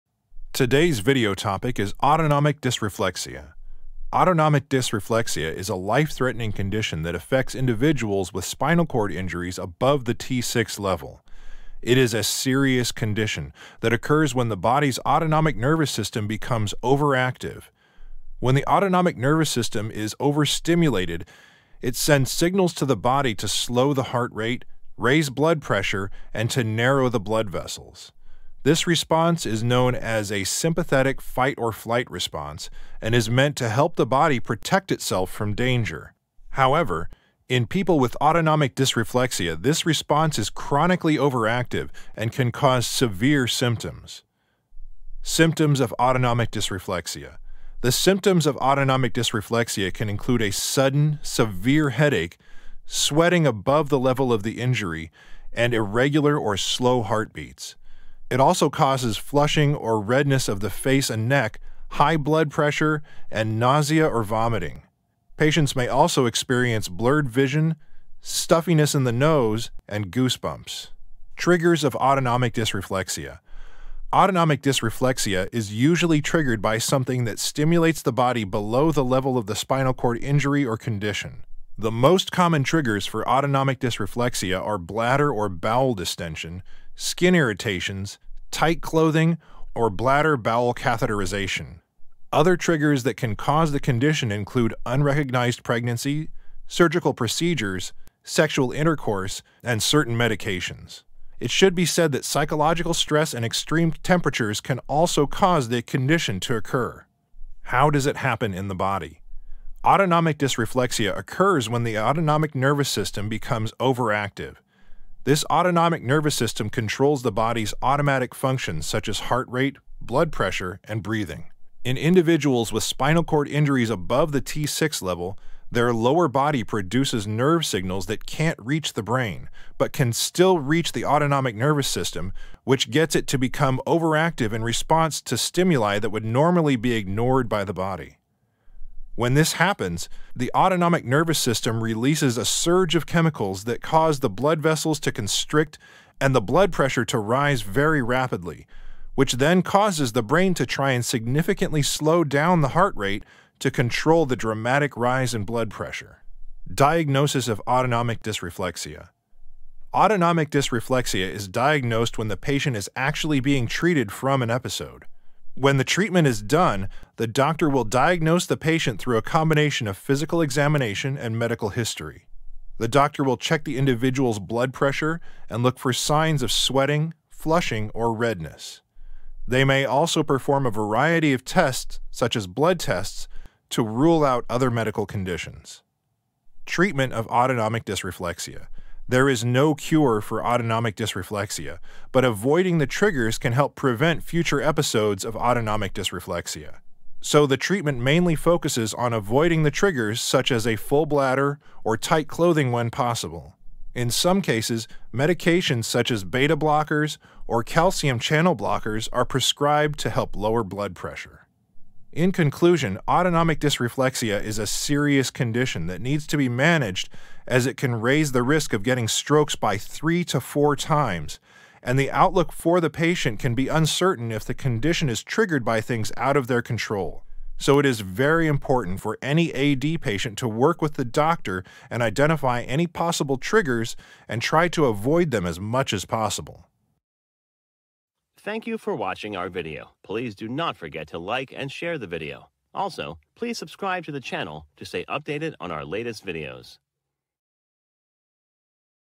Autonomic Dysreflexia: Navigating Life's Challenges with a Spinal Cord Injury
Chapters

0:00 Introduction
0:46 Symptoms of Autonomic dysreflexia
1:12 Triggers of Autonomic Dysreflexia
2:37 Diagnosis of Autonomic Dysreflexia
3:08 Treatment of Autonomic Dysreflexia

Autonomic Dysreflexia: The Hidden Dangers You Need to Know About

Autonomic dysreflexia is a potentially life-threatening condition that can occur in people with spinal cord injuries (SCI) at or above the T6 level. It is a medical emergency that requires immediate attention and treatment to prevent serious complications. In this blog, we will explore what autonomic dysreflexia is, its causes, symptoms, and treatment options.

What is Autonomic Dysreflexia?

Autonomic dysreflexia (AD) is a sudden and dangerous increase in blood pressure that can occur in people with SCI at or above the T6 level. The condition happens when there is an abnormal reflex response to a stimulus below the level of the injury, causing a sudden and excessive activation of the sympathetic nervous system. This, in turn, leads to an increased heart rate, sweating, and constriction of blood vessels, resulting in a sharp increase in blood pressure.

Causes of Autonomic Dysreflexia

The most common causes of autonomic dysreflexia are bladder and bowel problems, such as bladder distention, urinary tract infections, constipation, and rectal impaction. Other potential triggers include skin irritations, pressure sores, bone fractures, and other medical conditions.

Symptoms of Autonomic Dysreflexia

Symptoms of autonomic dysreflexia can vary from person to person, but the most common signs include severe headache, high blood pressure, sweating, flushing of the skin, goosebumps, and a sense of anxiety or panic. Other symptoms may include nausea, blurred vision, and nasal congestion.

Treatment Options

Autonomic dysreflexia is a medical emergency that requires prompt attention and treatment. The first step is to remove the stimulus that is causing the condition, such as catheter blockages, tight clothing, or pressure sores. This can often resolve the symptoms within minutes. If the symptoms persist, medication may be necessary to lower blood pressure.

Preventing Autonomic Dysreflexia

The best way to prevent autonomic dysreflexia is to identify and manage the triggers that can cause the condition. This includes maintaining good bladder and bowel management, preventing skin irritations and pressure sores, and seeking prompt medical attention for any health concerns.

In conclusion, autonomic dysreflexia is a serious medical condition that can occur in people with spinal cord injuries. It is important to recognize the symptoms, understand the causes, and seek prompt medical attention if it occurs. With proper management and prevention, people with SCI can reduce their risk of developing this life-threatening condition and lead a healthy and fulfilling life.